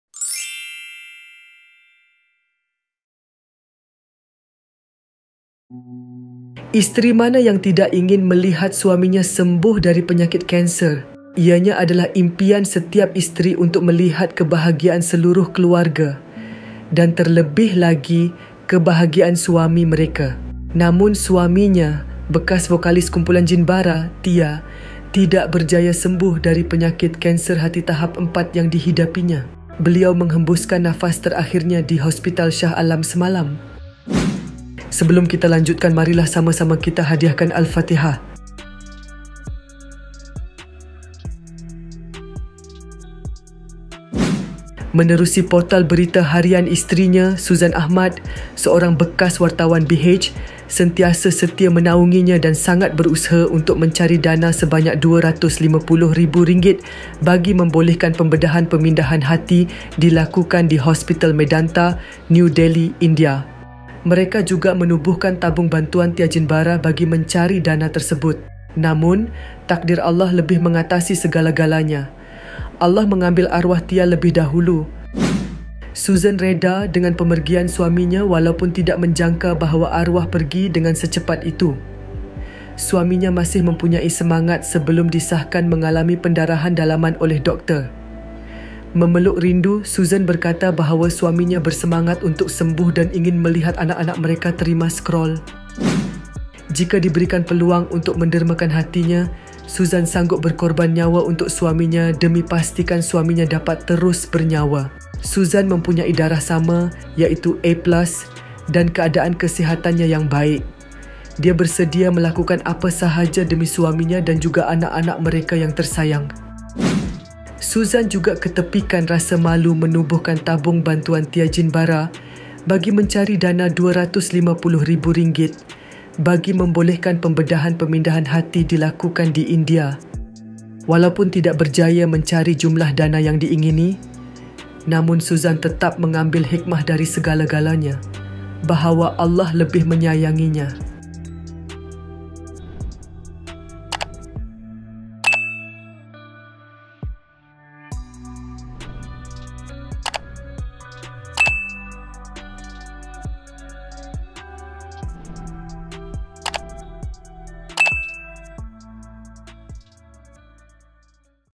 ISTERI SANGGUP Untuk Dermakan Hatinya Sendiri! Namun Allah Lebih Menyanyangi Tia Jimbara! Takziah!
Isteri mana yang tidak ingin melihat suaminya sembuh dari penyakit kanser? Ianya adalah impian setiap isteri untuk melihat kebahagiaan seluruh keluarga dan terlebih lagi, kebahagiaan suami mereka. Begitulah hasrat Suzan Ahmad, namun Allah lebih menyanyangi suaminya, bekas vokalis Kumpulan Jinbara, Tia.